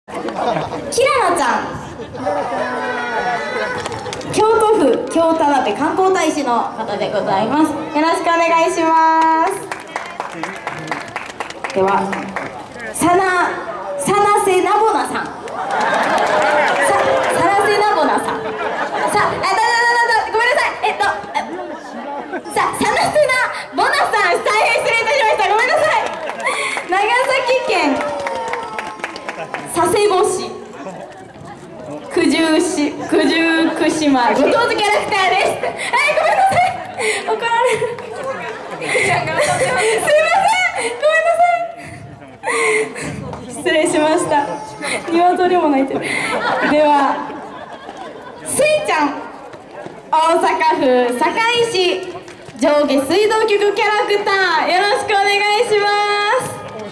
170625_さなせなぼな[01]区切るとこ違う
交野いきいきマルシェおりひめの駅「ぶどうマルシェ」withキャラパinKATANOで、交野市立総合体育施設「いきいきランド交野」に登場した『さなせなぼな』。名前の切るところを間違えられる。